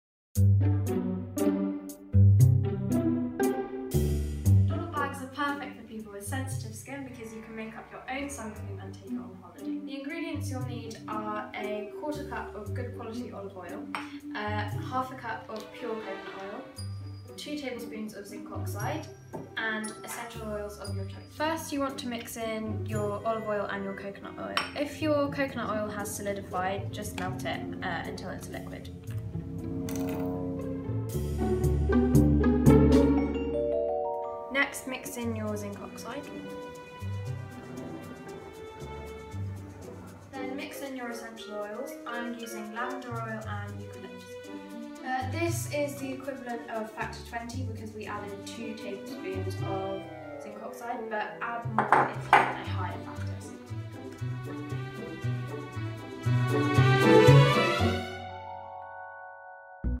How to make natural homemade suncream
Making your own skin friendly money saving sun cream is so easy you'll wonder why you didn't make it before. Fill the sunscreen in your DoddleBags and travel far and wide!